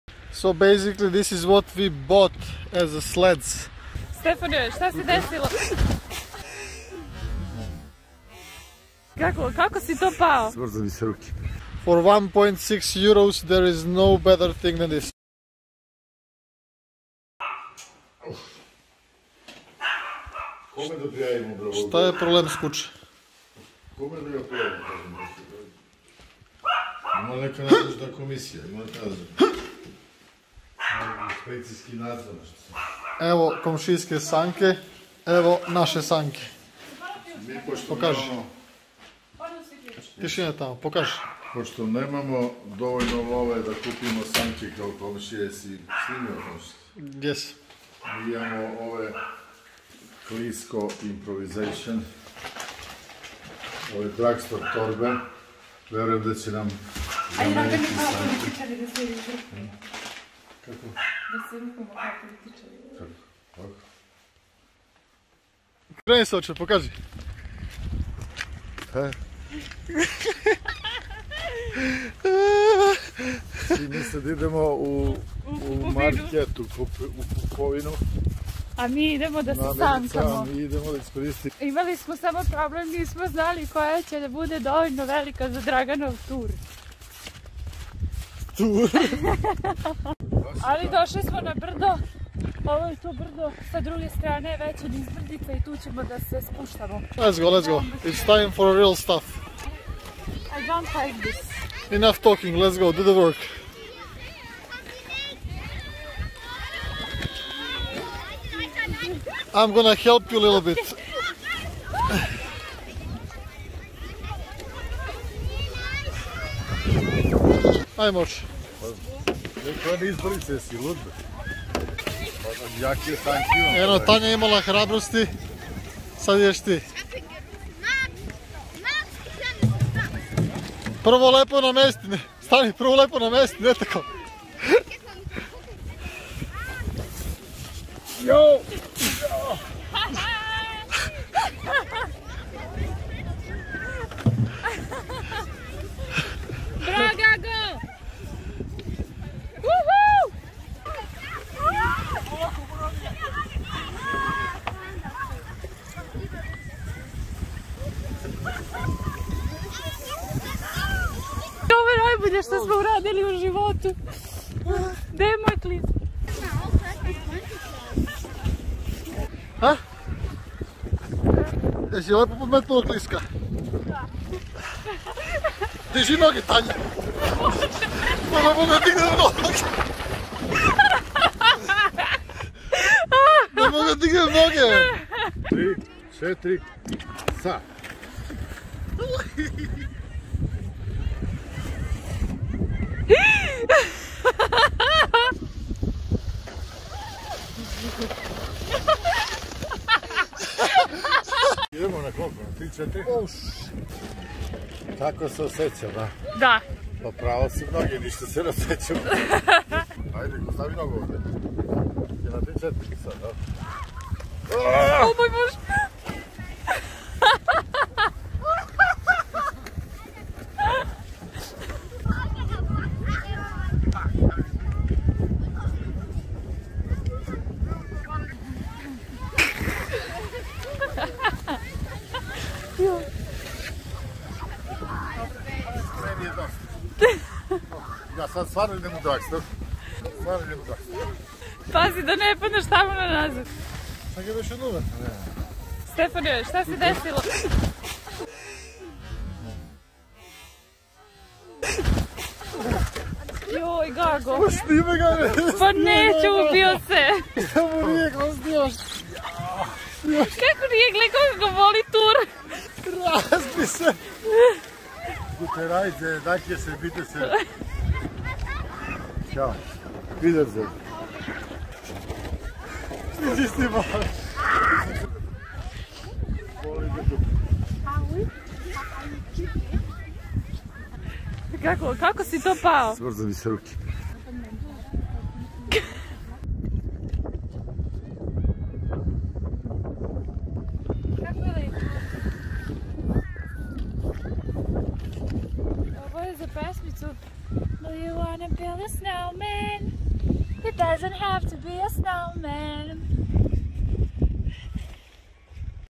sledding on shopping bags | vlog
Just a normal day for us! We ended up with a few bumps and bruises, but totally worth it haha of course, this adventure wouldn't be as half as funny if Dragan wasn't around - our mega star!

xoxo T